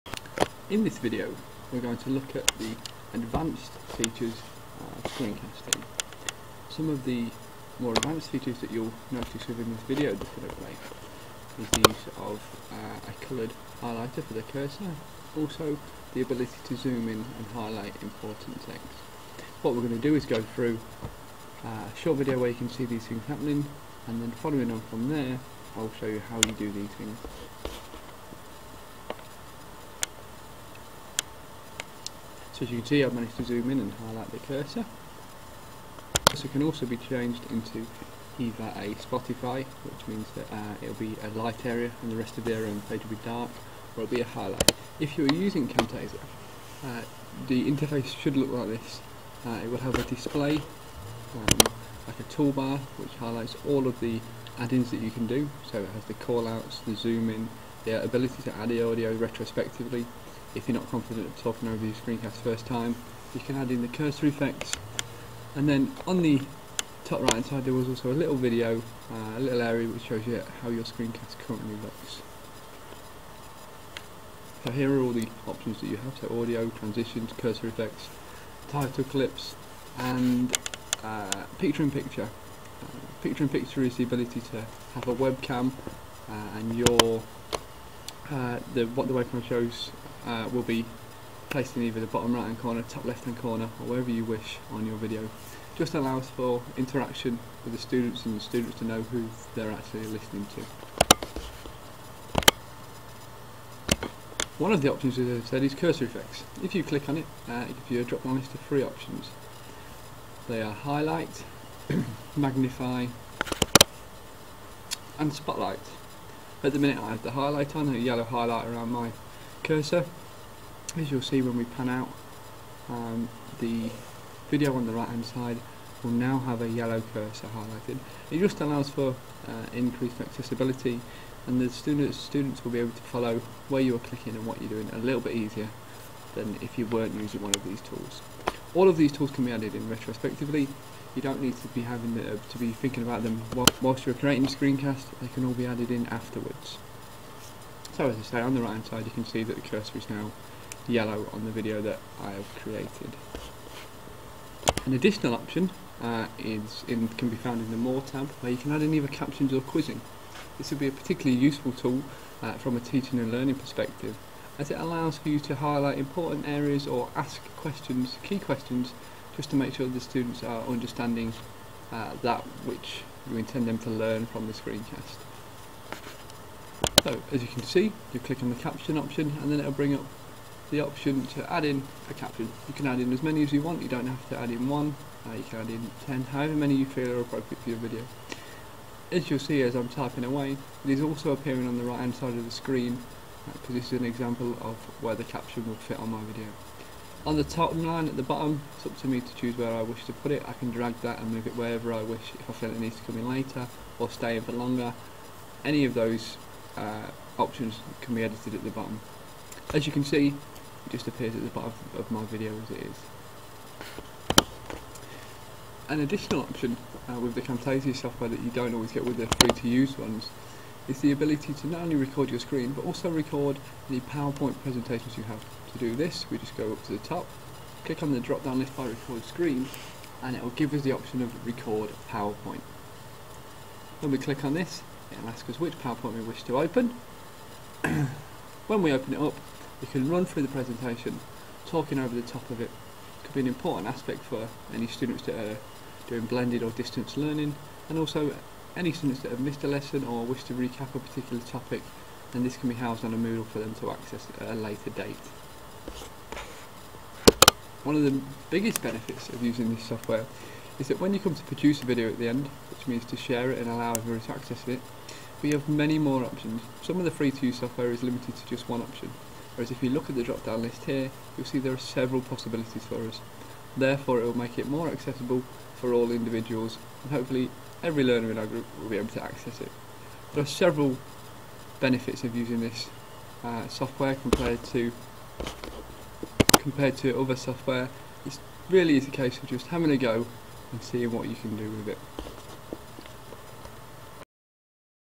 Advanced features of Screencasting
Screencast showing the advanced features of screencasting software. This was produced by Loughborough College as part of a LSIS funded project, on Improving teaching and learning through technology.